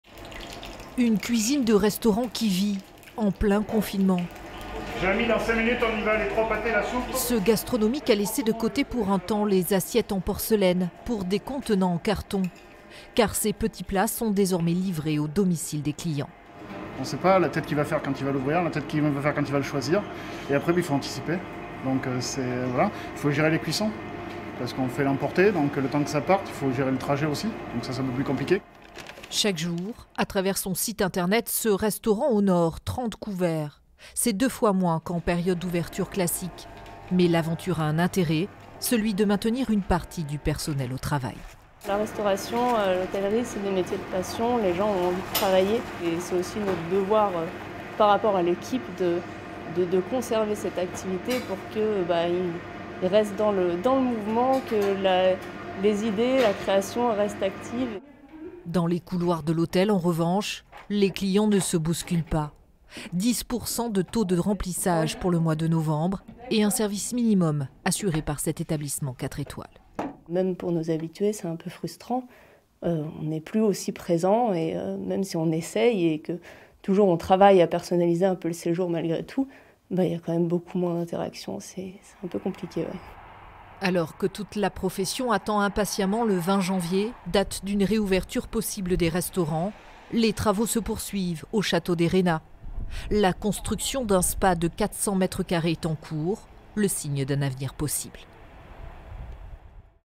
Gastronomie : le château des Reynats s'adapte au confinement
Reportage d'Emilie Bersars et Camille Michelland. Montage Alexandra Cassar.